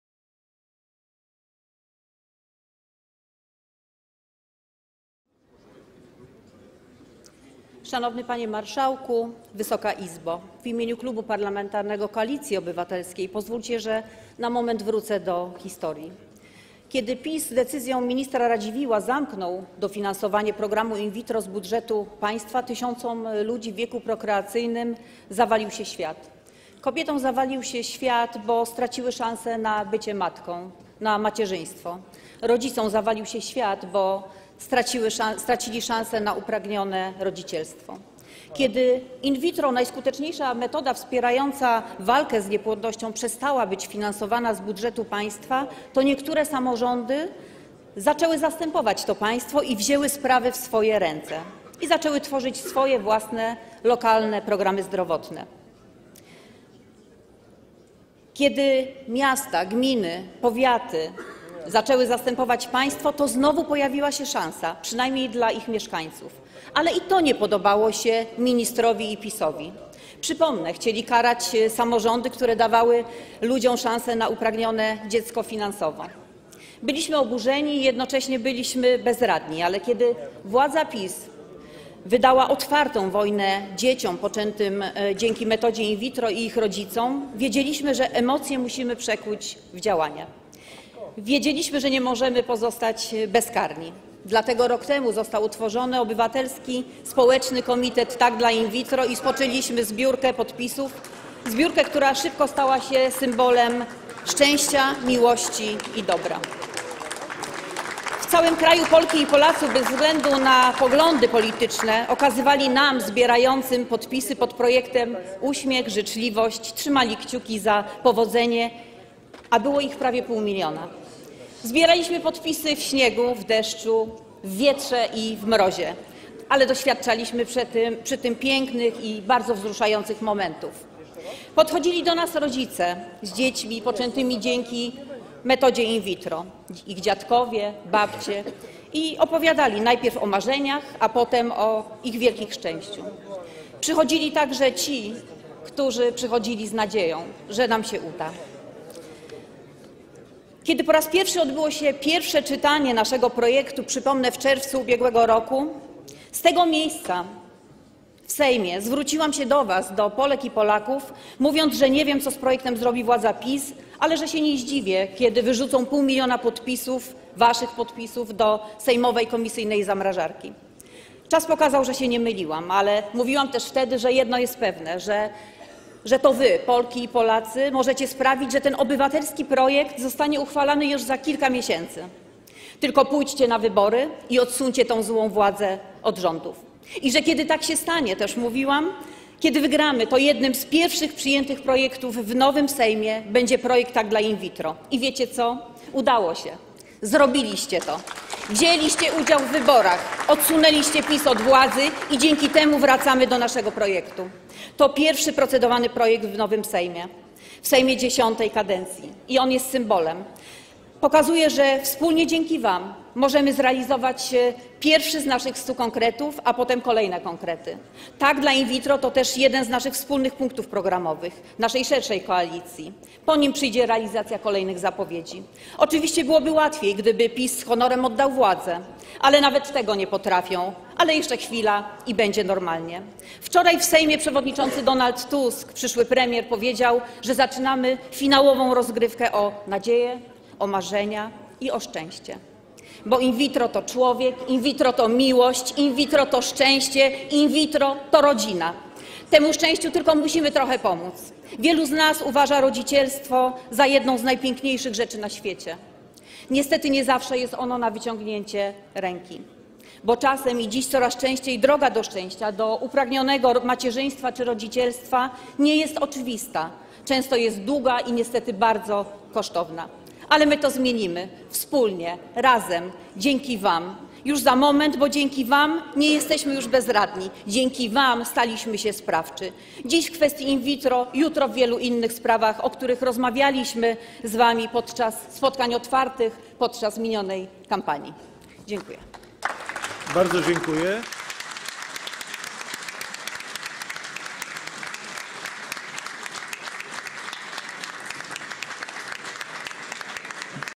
Monika Wielichowska - refundacja In vitro
Monika Wielichowska - refundacja In vitro [22 listopada 2023 r.]
1. posiedzenie Sejmu X kadencji

Pierwsze czytanie obywatelskiego projektu ustawy o zmianie ustawy o świadczeniach opieki zdrowotnej finansowanych ze środków publicznych